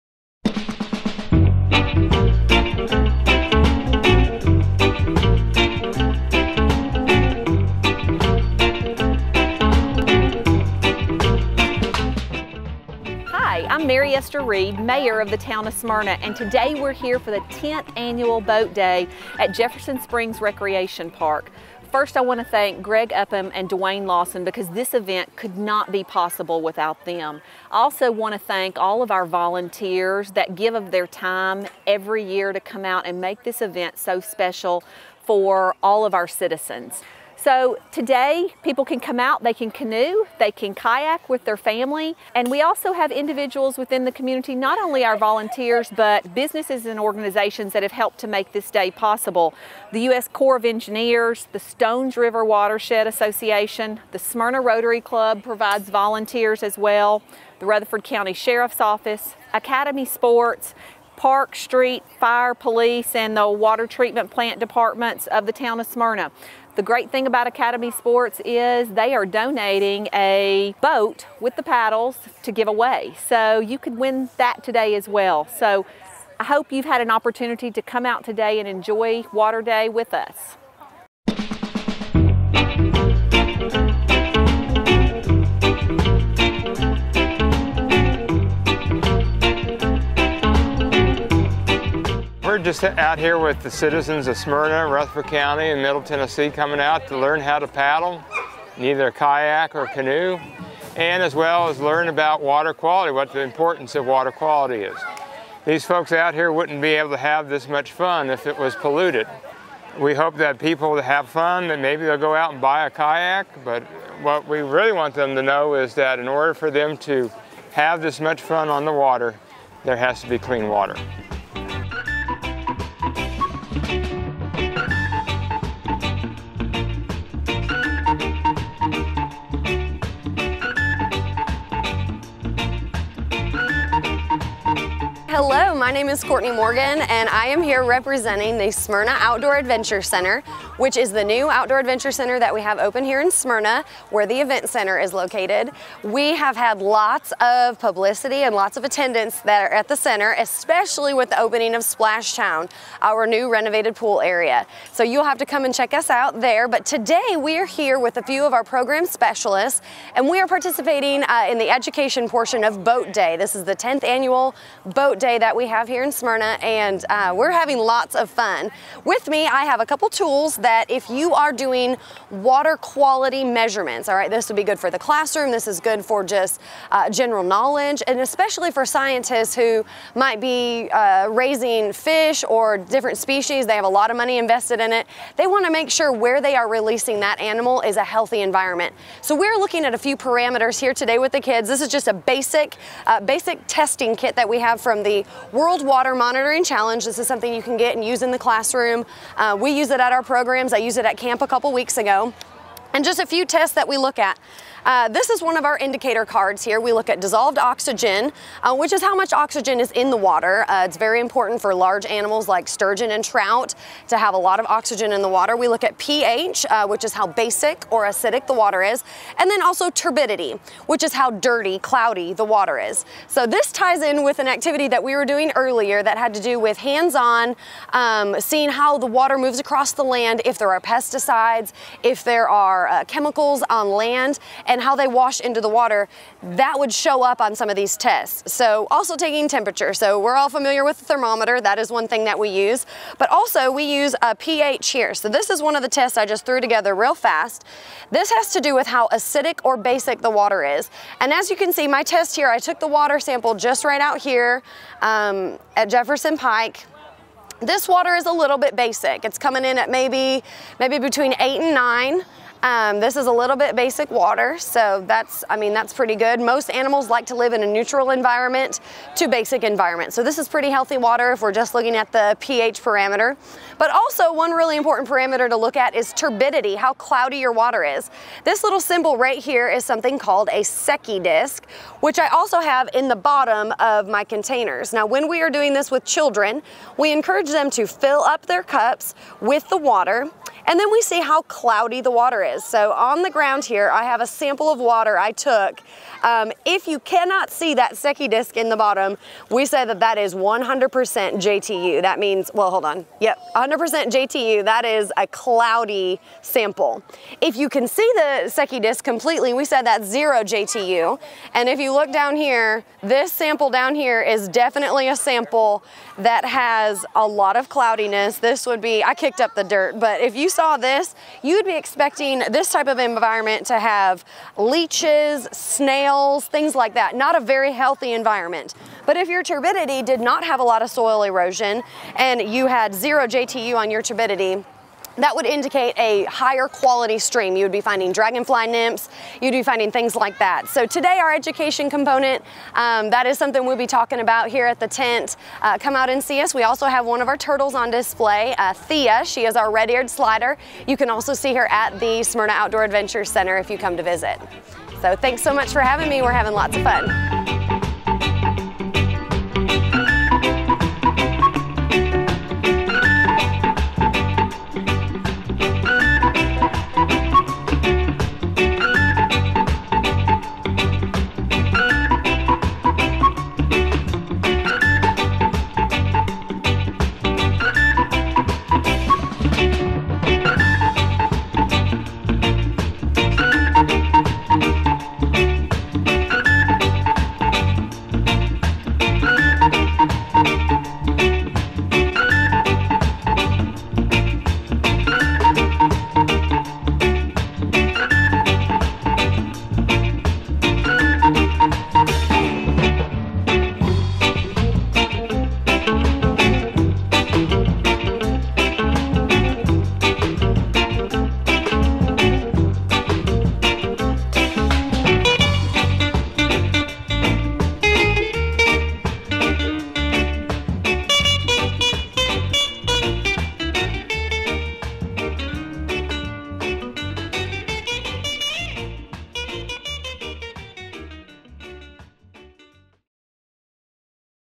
Boat Day 2017
The Town of Smyrna's 10th Annual Boat Day Event was held on Saturday, June 17th. 300 people enjoyed time on the water with the supplied kayaks and canoes.